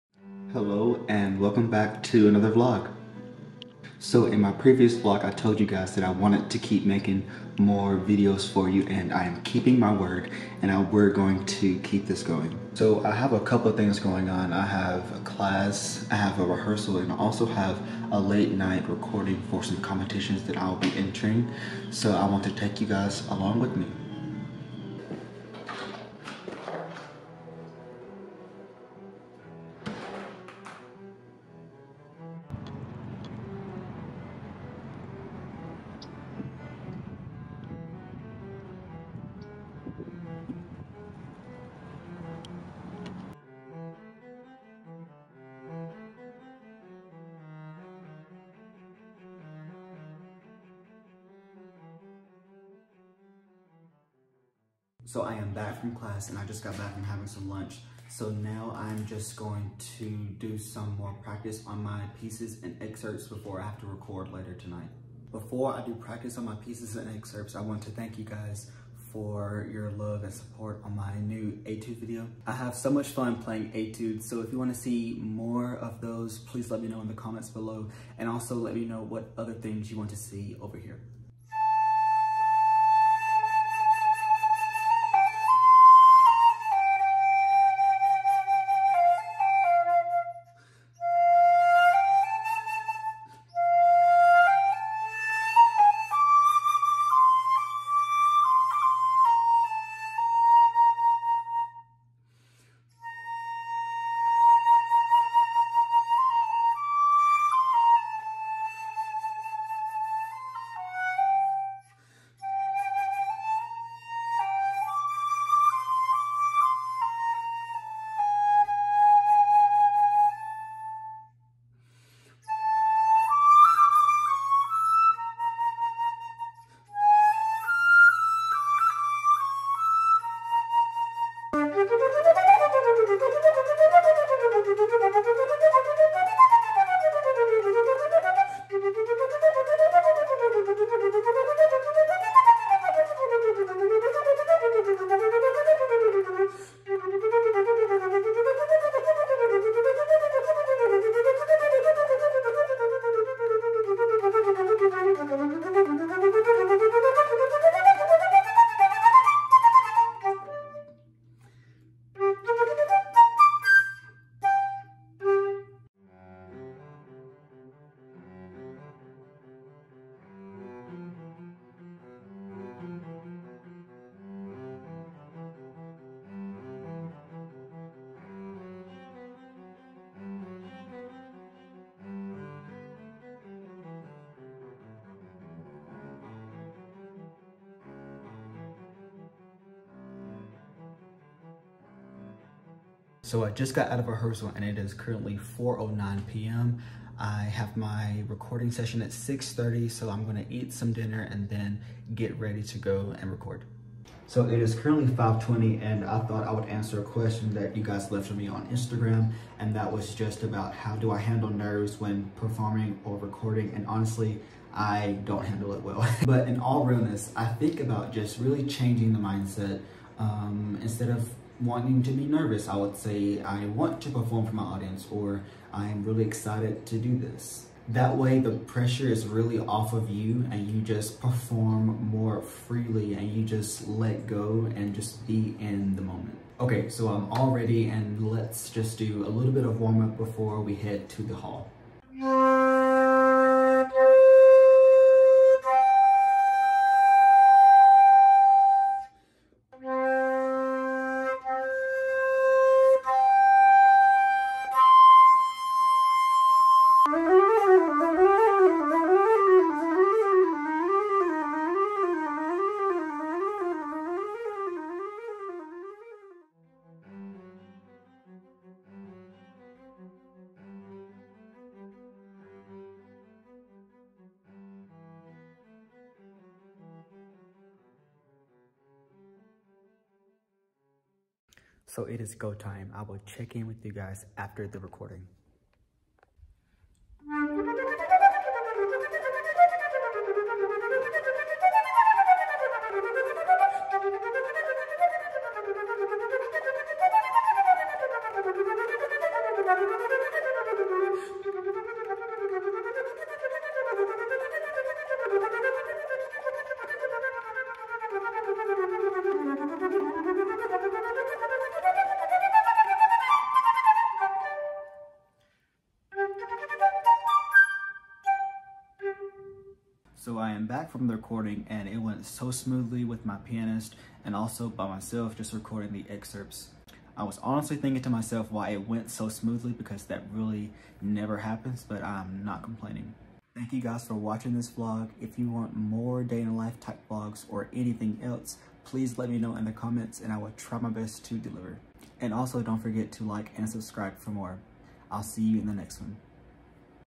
A Day in my Life: practicing, rehearsals, recordings| vlog
In this vlog: come with me to my first class of the day, rehearsal, and even my recordings